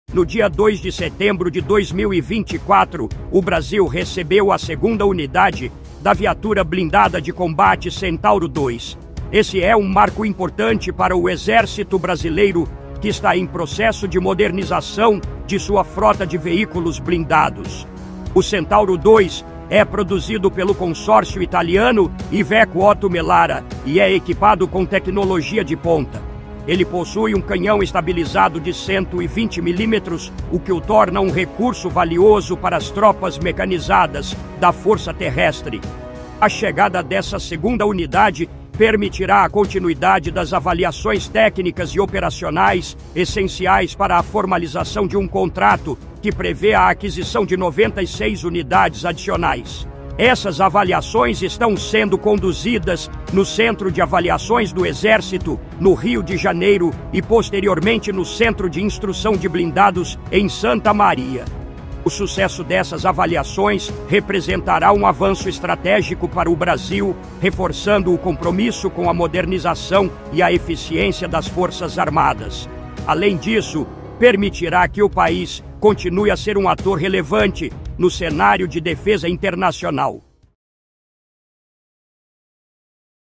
Centauro II: Mais um blindado chega ao Brasil
Olá, pessoal! Bem-vindos ao nosso canal! Hoje temos uma notícia incrível para compartilhar com vocês. O segundo blindado Centauro II acaba de chegar ao Brasil! Vamos conferir todos os detalhes dessa novidade que promete fortalecer ainda mais nossas Forças Armadas.